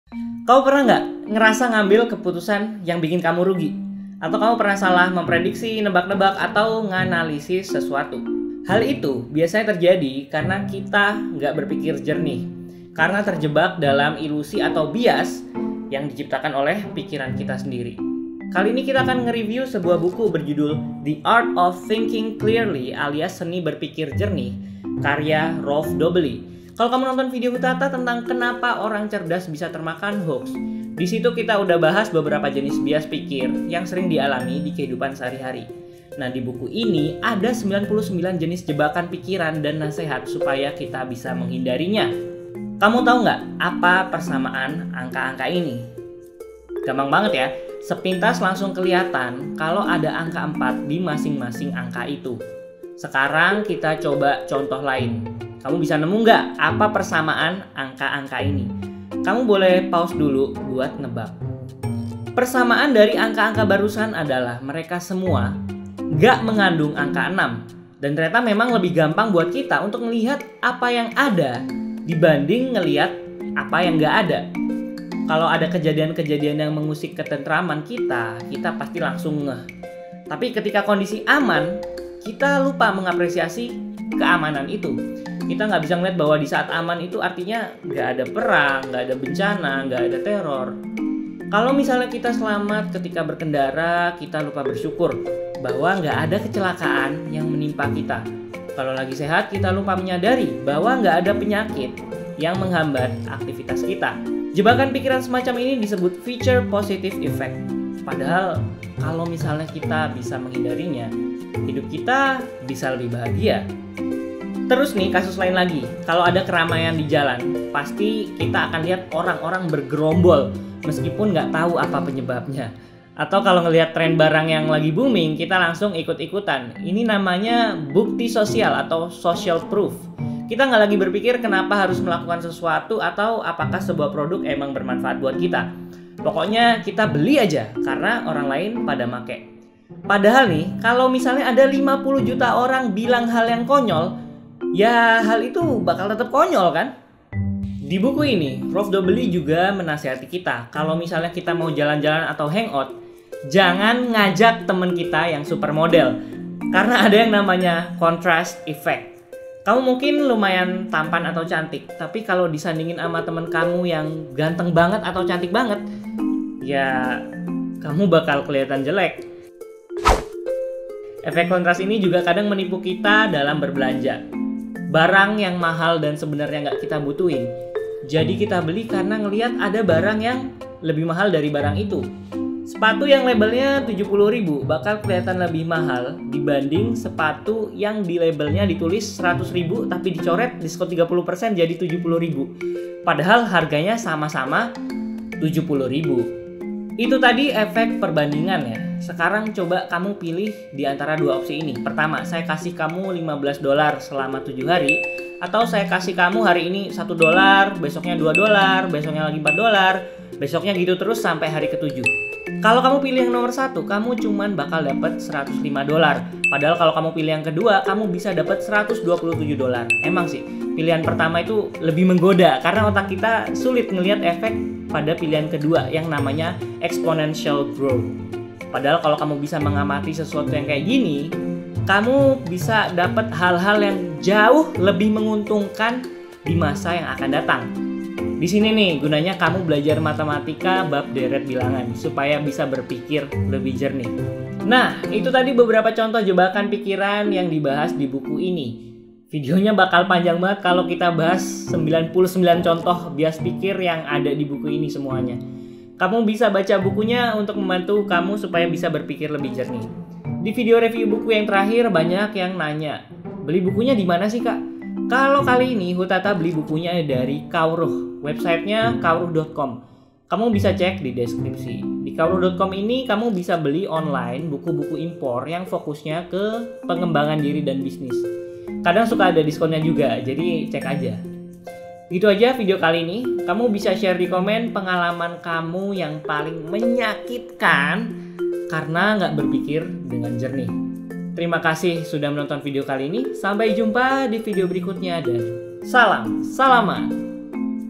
Cara Berpikir dengan Jernih
Kamu pernah merasa mengambil keputusan yang bikin kamu rugi? Atau kamu pernah salah memprediksi, nebak-nebak, atau menganalisis sesuatu? Di video kali ini, kita review buku yang membahas cara berpikir yang jernih biar kamu bisa membuat keputusan sebaik mungkin.

HUTATA

Kita membahas pertanyaan-pertanyaan random yang terkadang membuat kita tidak bisa tidur di malam hari.

Mau tahu lebih jauh tentang Hujan Tanda Tanya?